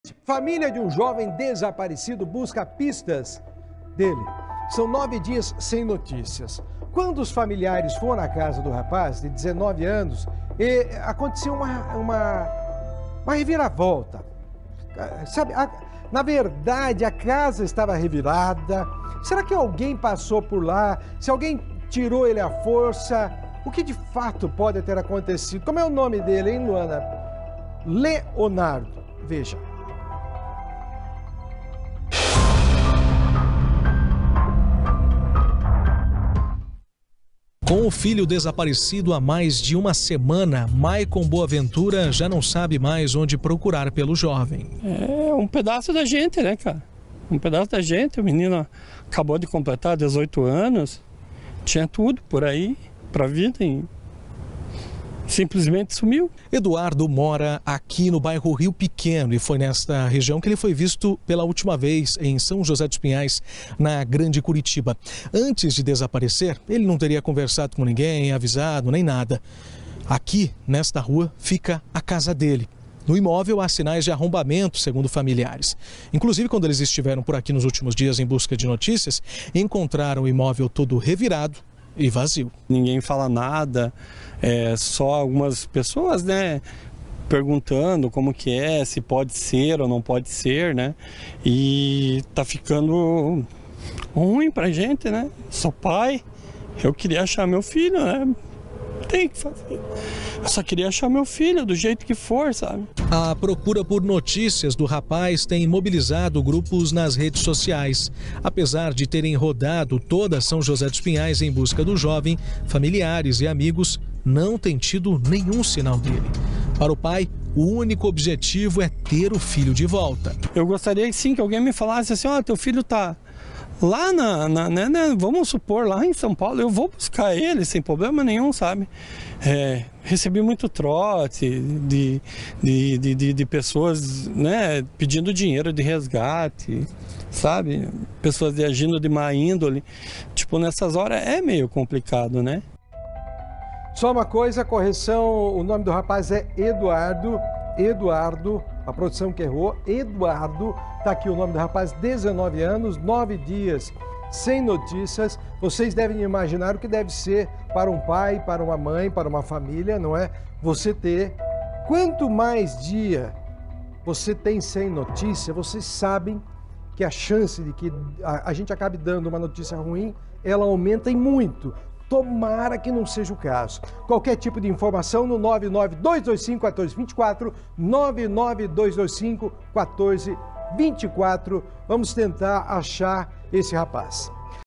Pai busca pistas do paradeiro do filho que está desaparecido há 9 dias
Família de jovem desaparecido busca pistas do paradeiro. Já são 9 dias sem notícias, quando familiares foram a casa do rapaz, de 18 anos, ela estava revirada